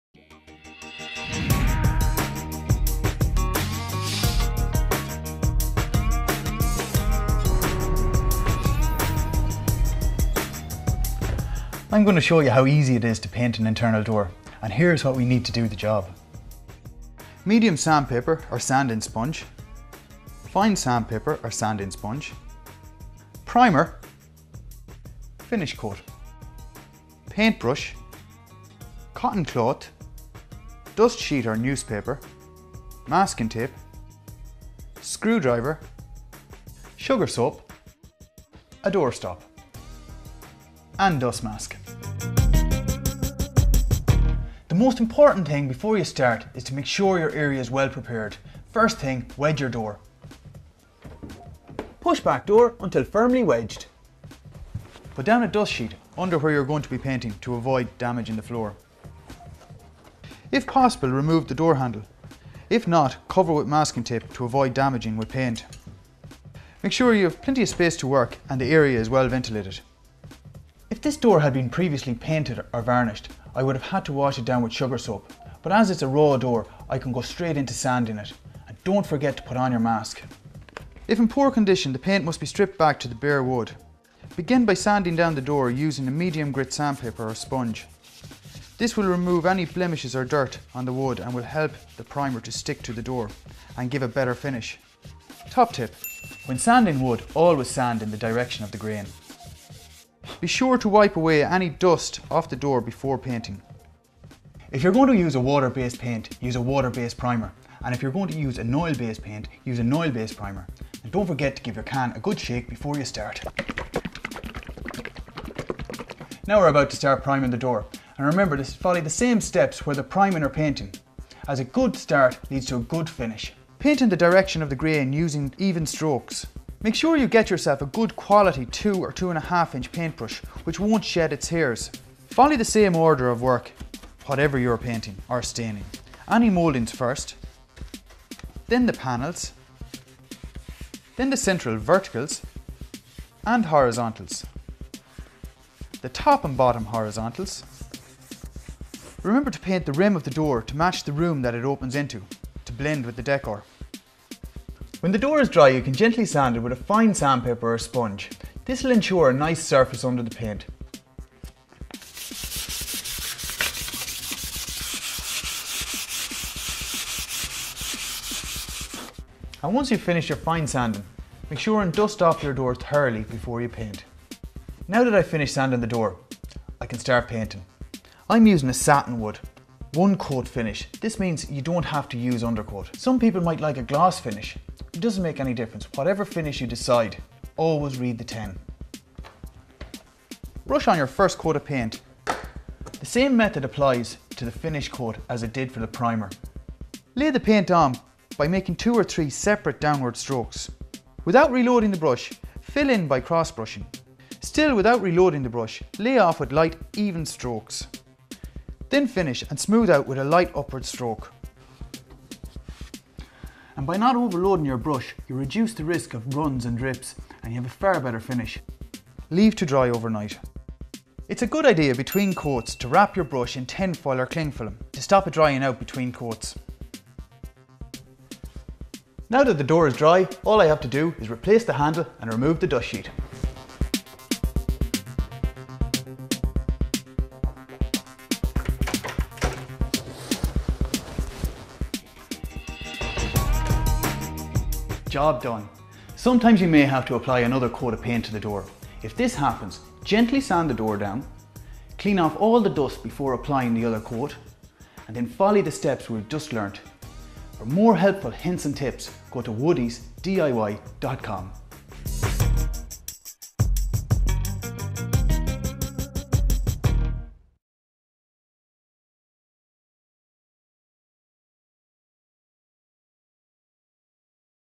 How to Paint an Interior Timber Door
Woodies will show you how to paint an interior wooden door.